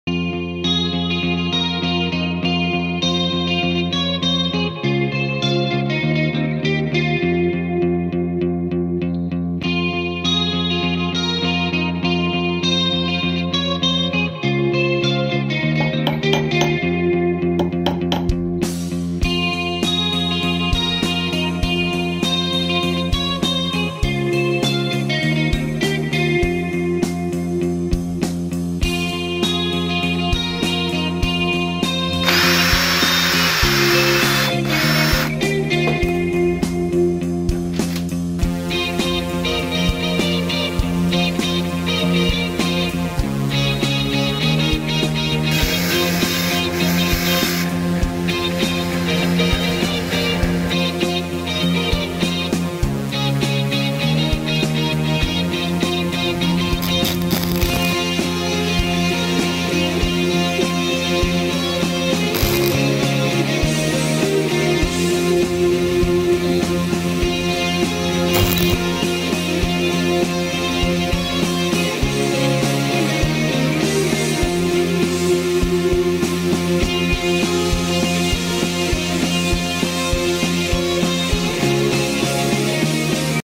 Queres aprender hacer un rifle de franco tirador con materiales en desuso??? ♻️
Acá les comparto el paso a paso como fui haciendo esta artesania y su ensamblado 😉
Espero que puedan hacer su propia obra.